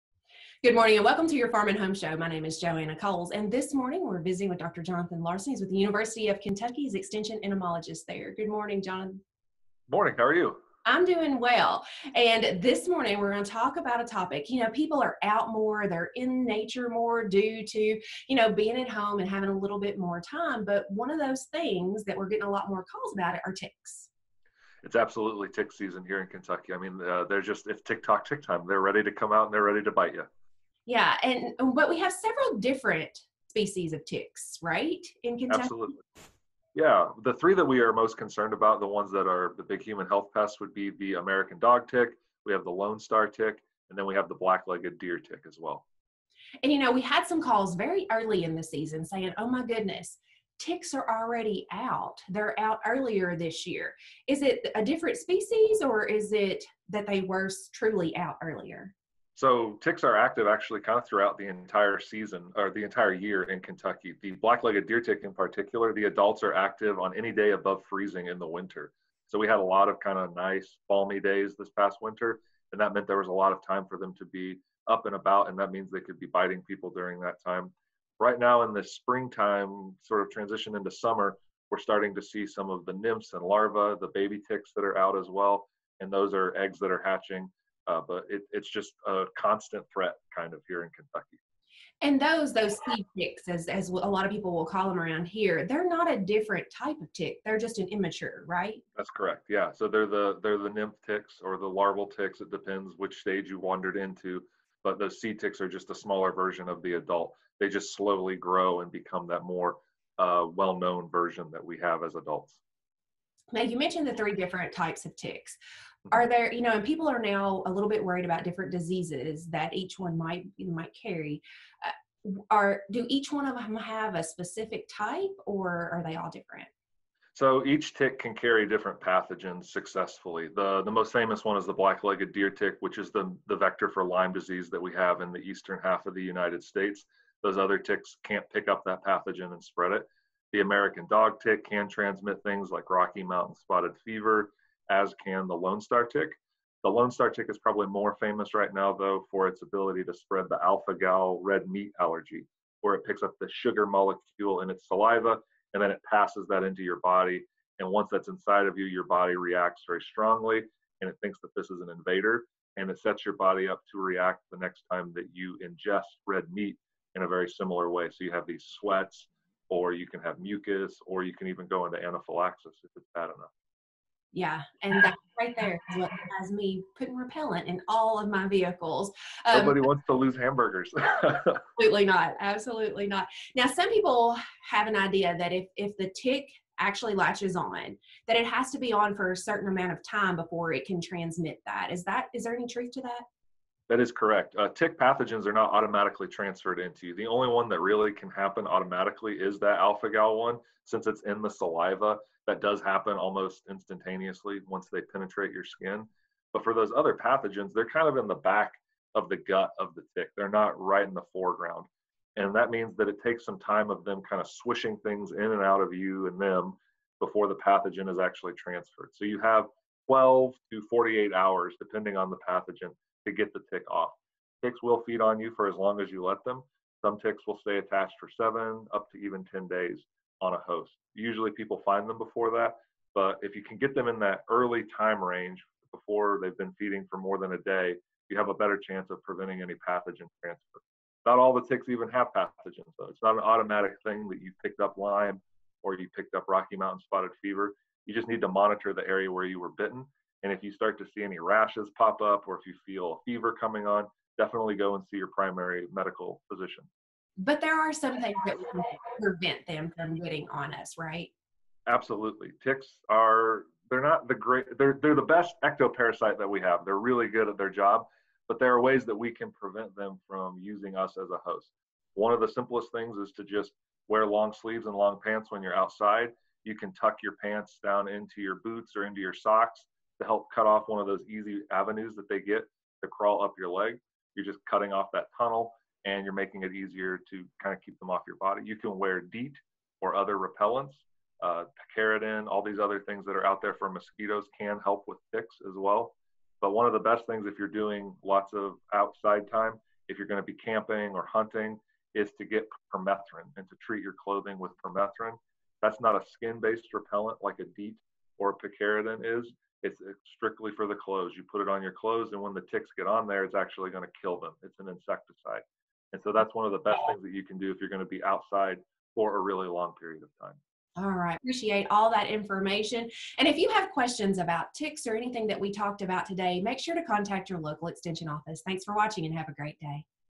Ticks in Kentucky
Joanna visits with Dr. Jonathan Larson, UK Extension Entomologist to talk about ticks in Kentucky.
In Kentucky, we have three species we are most concerned with, the American dog tick, the lone star tick, and the black-legged deer tick. All three can transmit pathogens that can cause disease. You have time to remove a tick before a pathogen is transferred. If you can get the tick off of you before they have fed for 12 hours, you should be safe. It's important to remember that not all ticks are infected as well! Ticks can be prevented by wearing long sleeves and pants, treating your skin with an insect repellent such as DEET, and using permethrin on your clothing. Ticks can be active all year in Kentucky. Right now we are seeing lots of lone star ticks and some dog ticks. In the winter the black-legged deer tick can be active on any day above freezing.